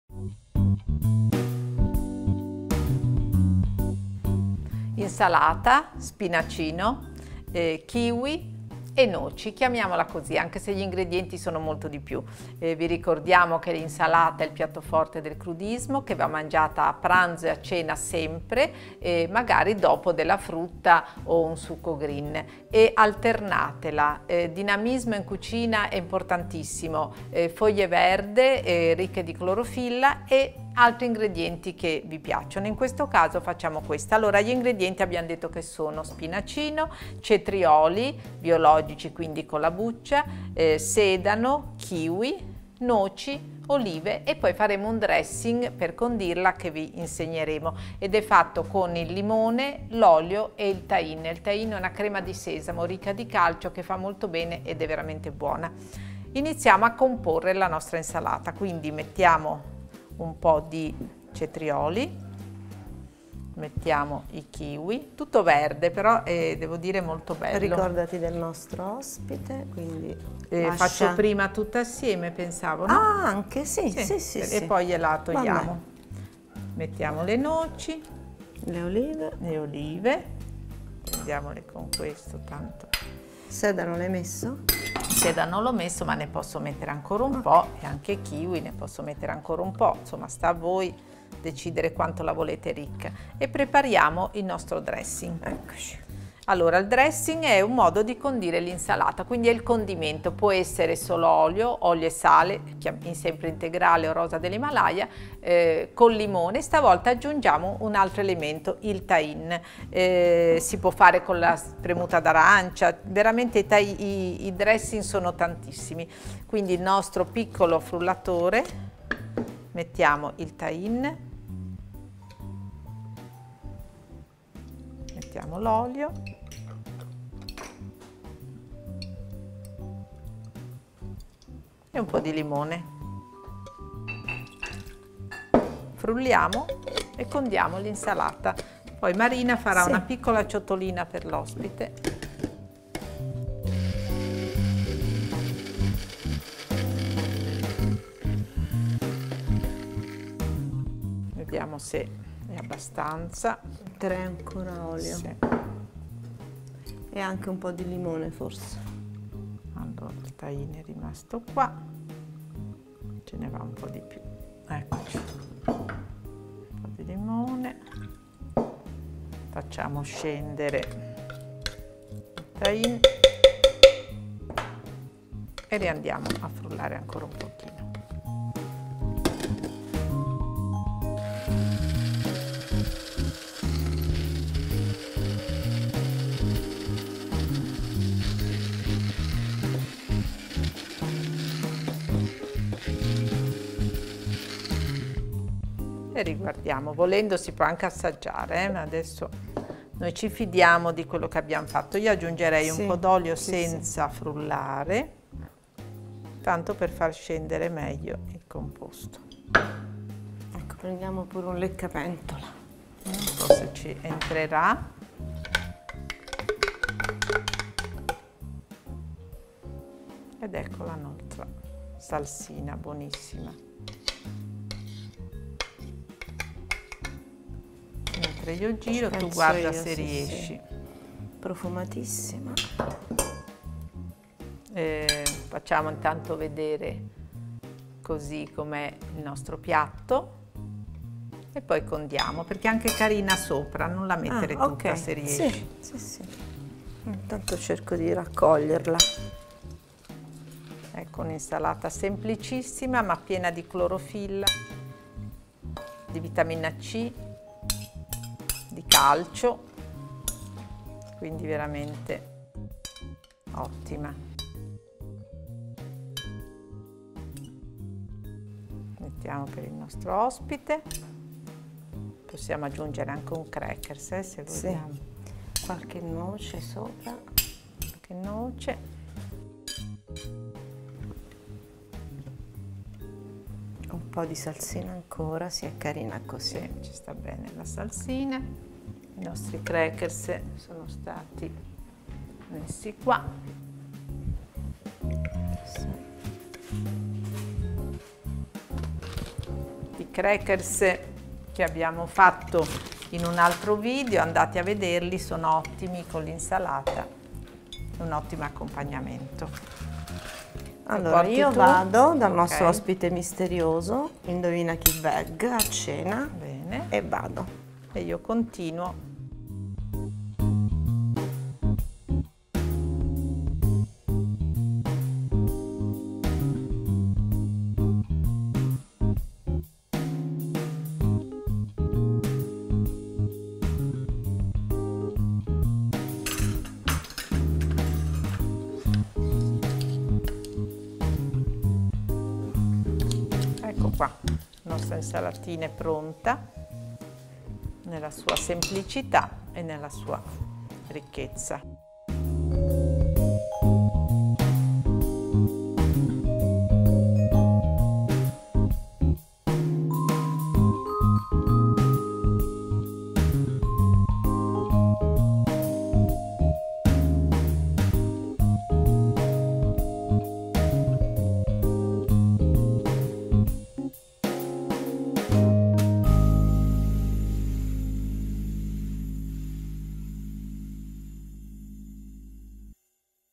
Il vegano per le feste - Insalata di songino, kiwi e noci
L'insalata è il piatto forte del crudismo: Laura e Marina hanno realizzato per te questa insalata ricca di clorofilla, vitamina C e di calcio, condita con un dressing al limone.
Vai ora al sito troverai questa e altre squisite ricette vegane e. buon appetito.

Insalata di songino, kiwi e noci
4 tazze di songino
2 kiwi
1 cetriolo
8 noci
2 tazze di olive

Sbuccia e taglia i kiwi.
Taglia il cetriolo con la mandolina.
Componi l’insalata in una ciotola, condisci a piacere e servi.

Dressing al limone
succo di 1 limone
4 cucchiai di olio extravergine d’oliva
1 cucchiaino di salsa tahin
sale rosa dell’Himalaya q.b.

Frulla tutti gli ingredienti per ottenere il dressing e condisci l’Insalata di songino, kiwi e noci.